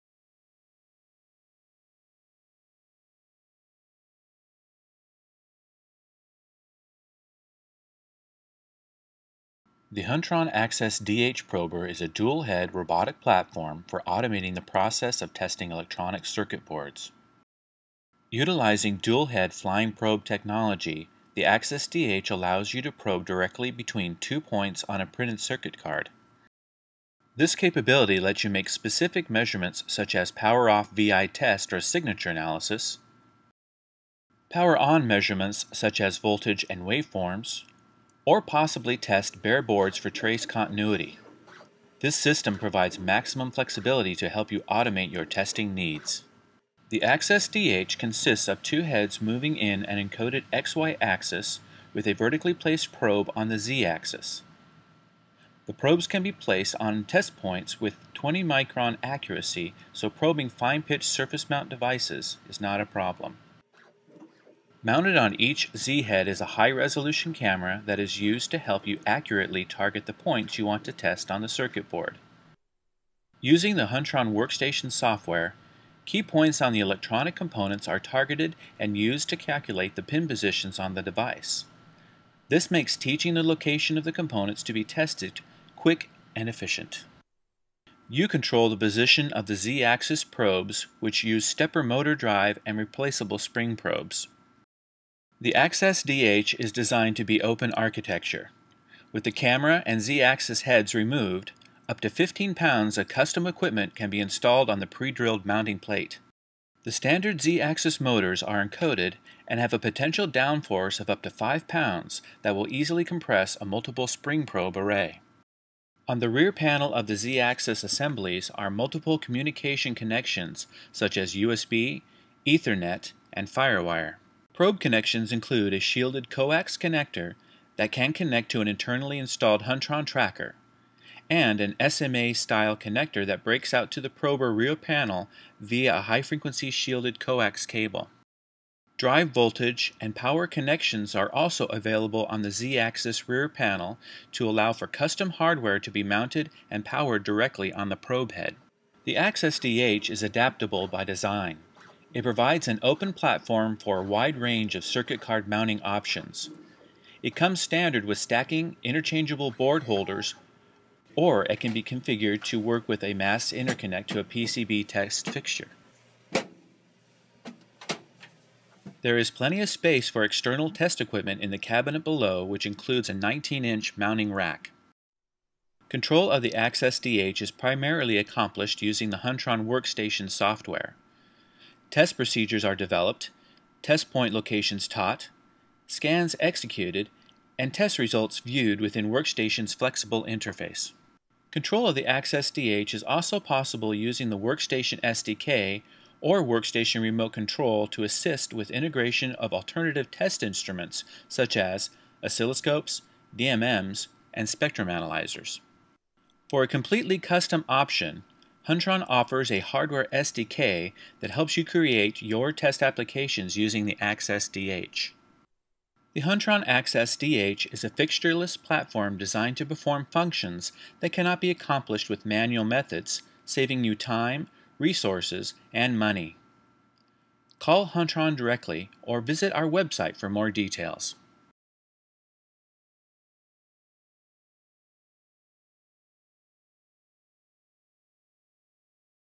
See updated Access DH2 demonstration - Huntron Access DH dual head Flying Probe Video
**This video highlights the first generation Huntron Access DH dual head flying prober. The next generation Access DH2 has been released and has a demonstration video on this channel.**
Flying probe technology is being used more extensively in the automated testing of printed circuit boards. This video provides a basic overview of the Huntron Access DH Robotic Prober. It uses dual flying probe heads to interface to points on a printed circuit board with 20 micron accuracy.

- Point to point measurements such as ASA
- Bare board continuity testing
- Mechanical procedures that require a robust XY robotic platform